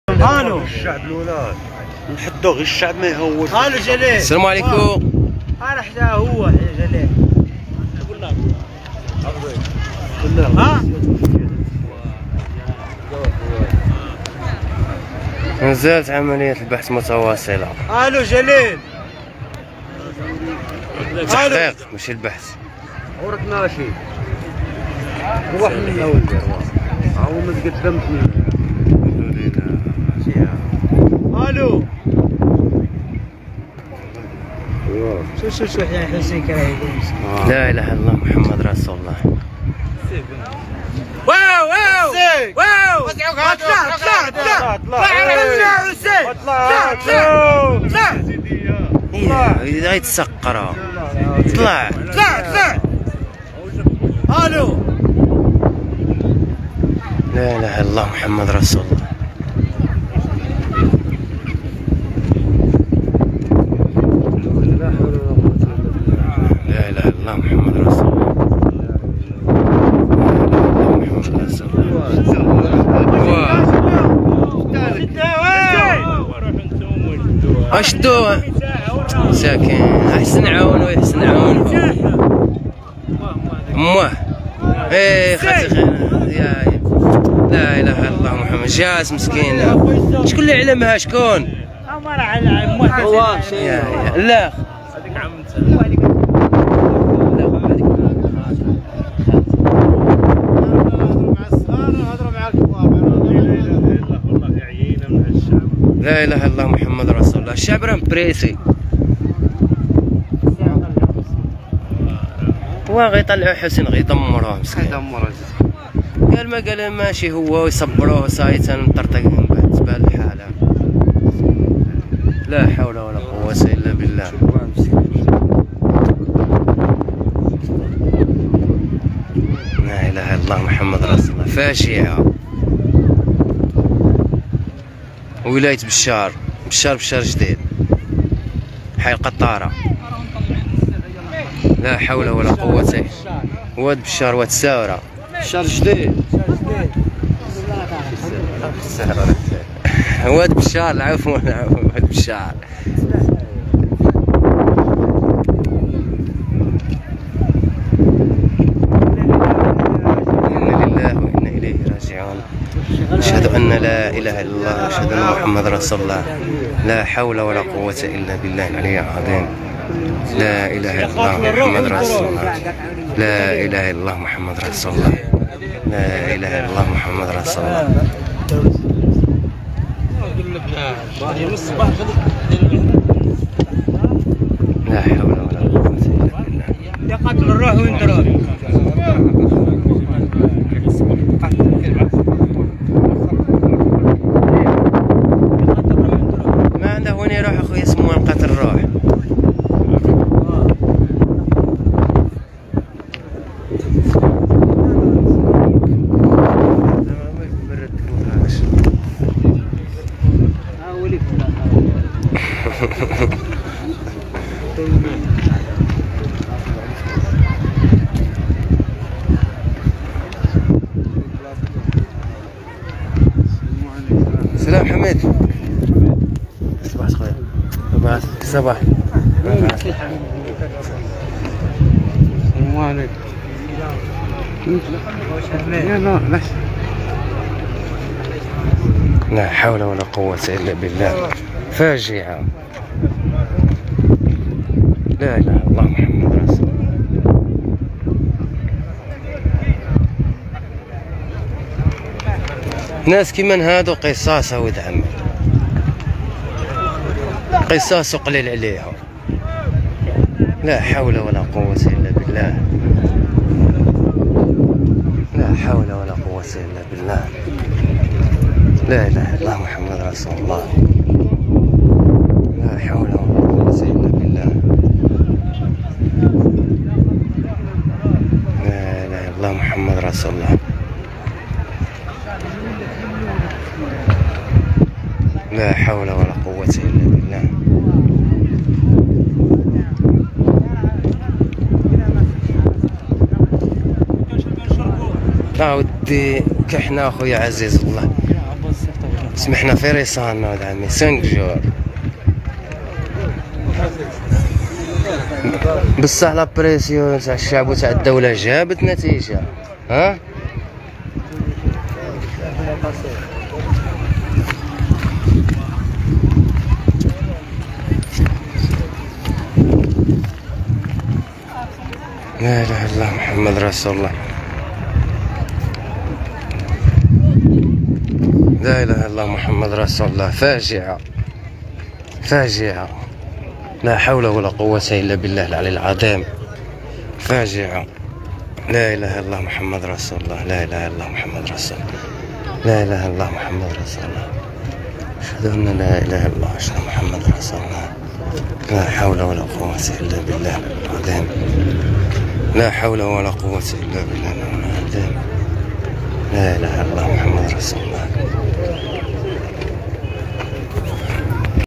ولاية بشار | العثور علي الطفل هوايشي صهيب
ولاية بشار العثور على الطفل هوايشي صهيب  في واد ببشار الجديدة
بعد بحث دام خمسة أيام تم العثور على الطفل هوايشي صهيب
ان لله وان اليه راجعون